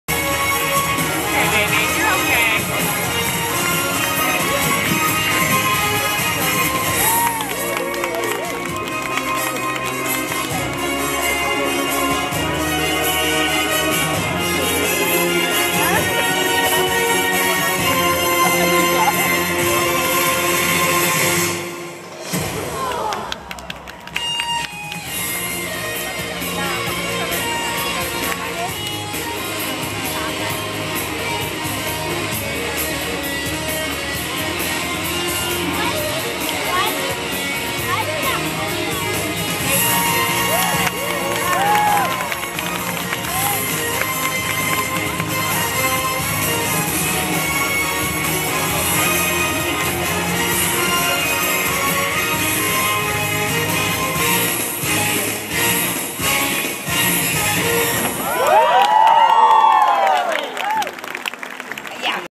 Hey Cameron! Cirque de Mer at Sea World San Diego
Even though I'm petrified of clowns... Cirque de Mer is a GREAT show and visually awesome display of acrobats. Highly Recommended by Hey Cameron! Blog.